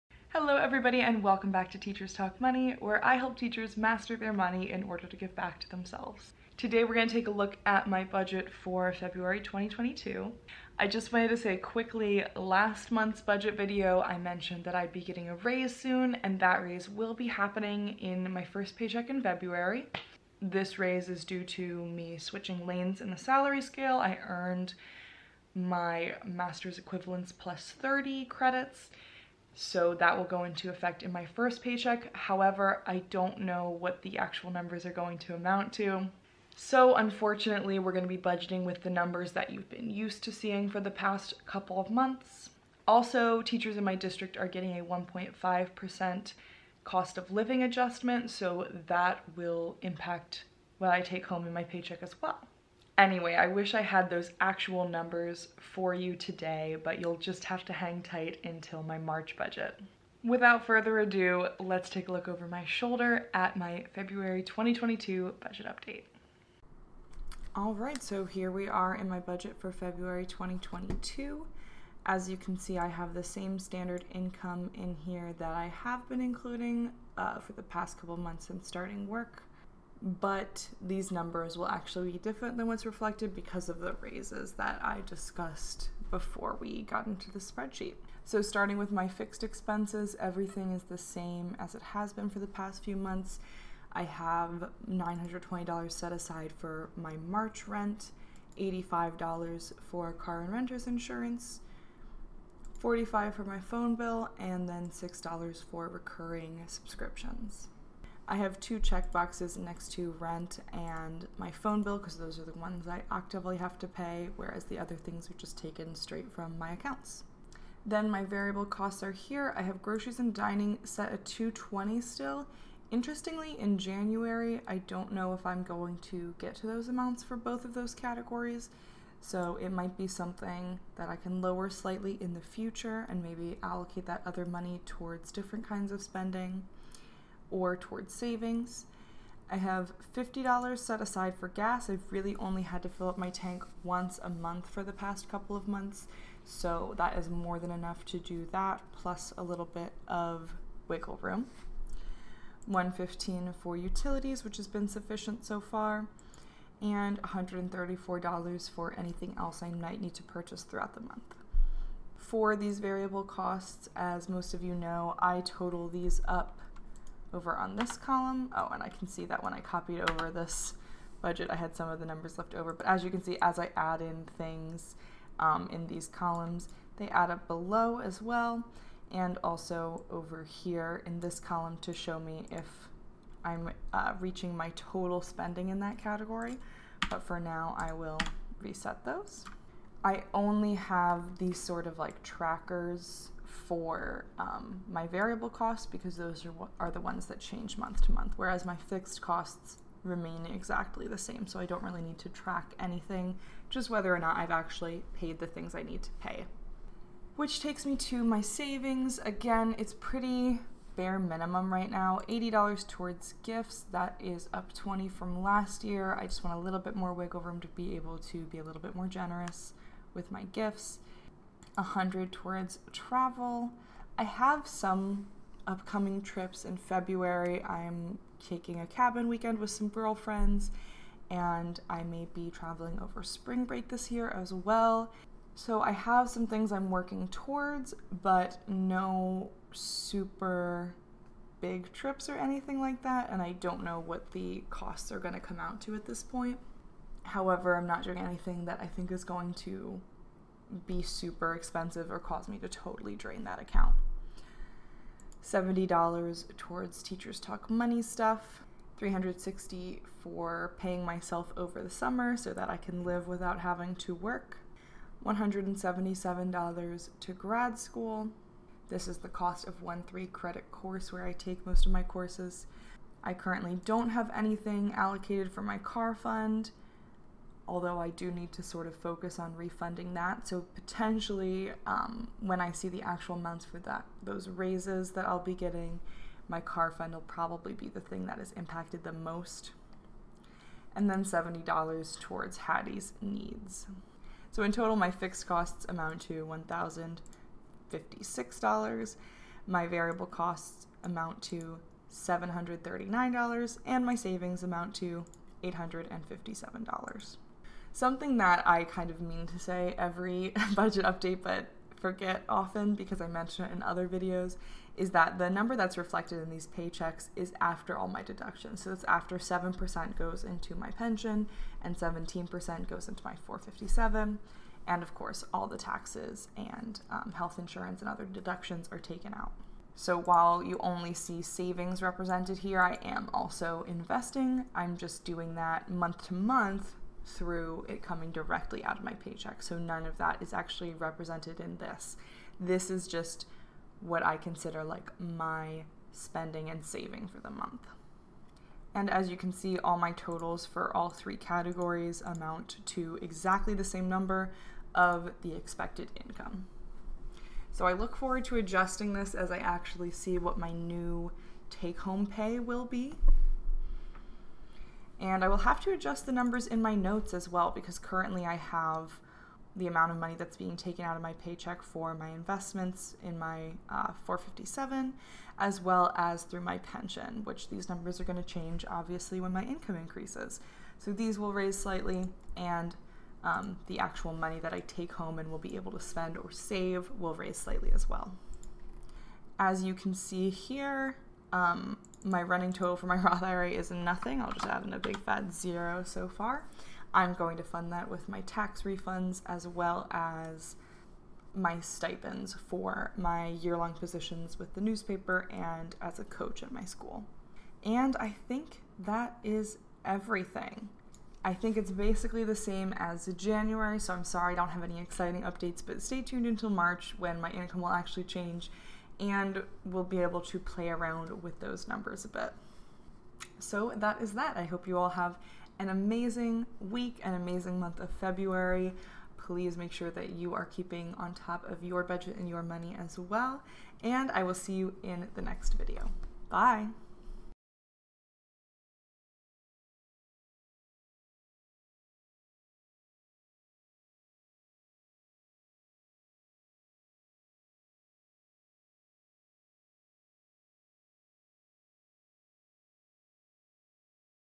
Budget with Me February 2022 | Teachers Talk Money
I'm still awaiting my raise from getting my Master's Equivalence +30 credits, as well as a 1.5% cost of living adjustment from my district (negotiated by my union). Because I don't know what my exact take-home pay will be, this budget is based on my current numbers-- stay tuned until March to see the new take home amounts & how they'll impact my budget.

If you’re ready to get started on your money journey, use my FREE resources below!